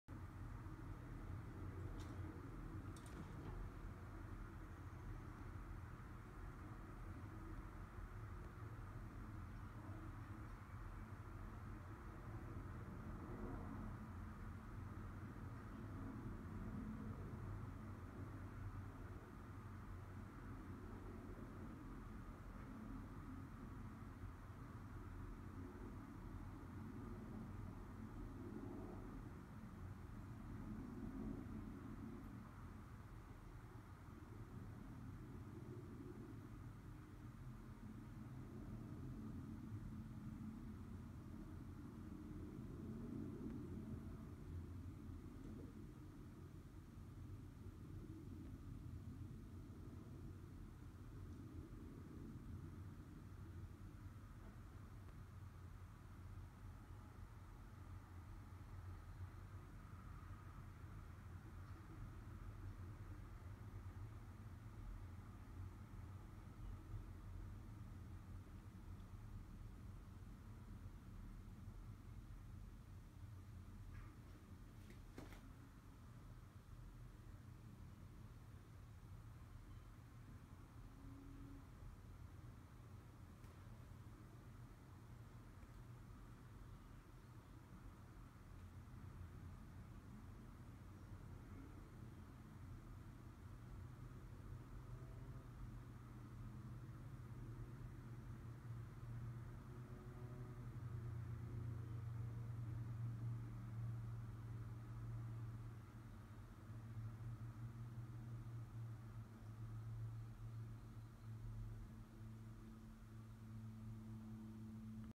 Peanut practices tough love at breakfast
Peanut teaches her baby that it is bad manners to interrupt another squirrel when they are eating.